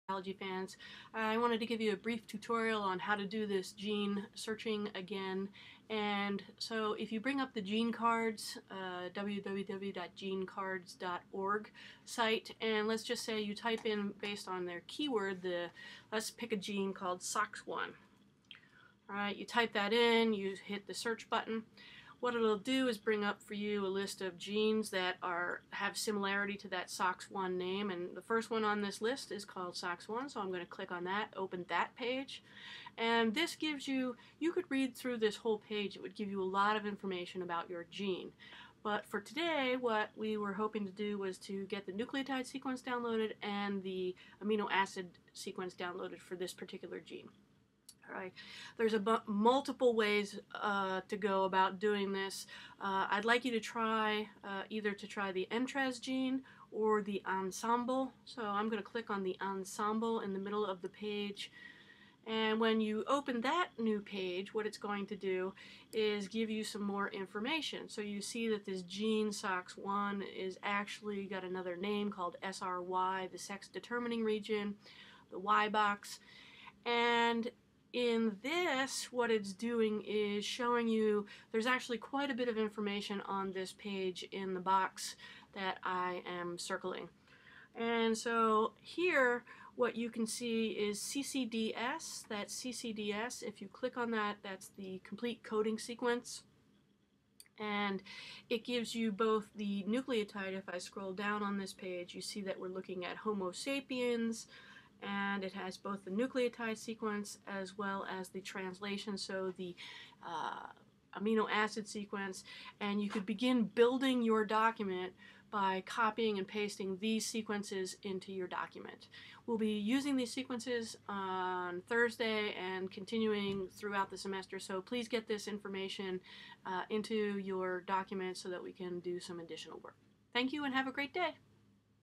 Finding Your Gene Information
Here, I give you a brief description of finding your gene in GENECARD and how to find the DNA and proteins sequences from there.